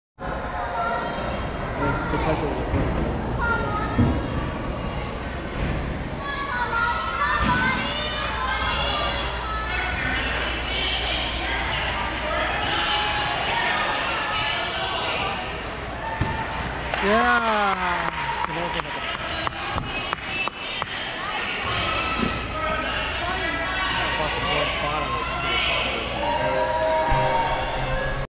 Lani De La O 3rd Place Bars World Elite Cup 2019 Wildfire Gymnast Level 5
3rd Place Bars 9.350. 3rd AA 36.375.